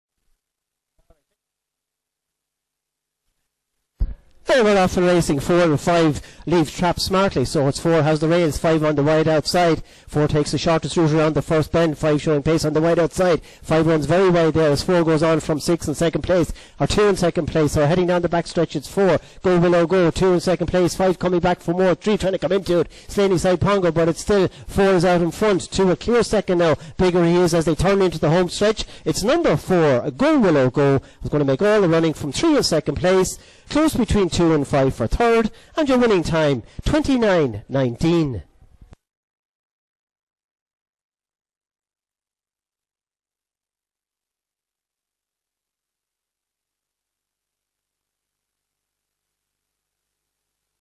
IGB - Paddy And Ciara Sharky Consolation Semi-Final 04/05/2019 Race 1 - Waterford
Race: Paddy And Ciara Sharky Consolation  Semi-Final
Track Name: Waterford
Date: 04/05/2019
Hour: 19:46

Results

1st    4     GO WILLOW GO        4/6f         29.49
2nd    3     SLANEYSIDE PONGO    3/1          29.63
3rd    2     BIGGER HE IS        5/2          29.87
4th    5     SIRIUS PRINCE       6/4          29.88
5th    6     MOULTON SWIFT       6/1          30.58
6th    1     BALLINCREA NELL     6/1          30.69

|WFD_2019_05_04_01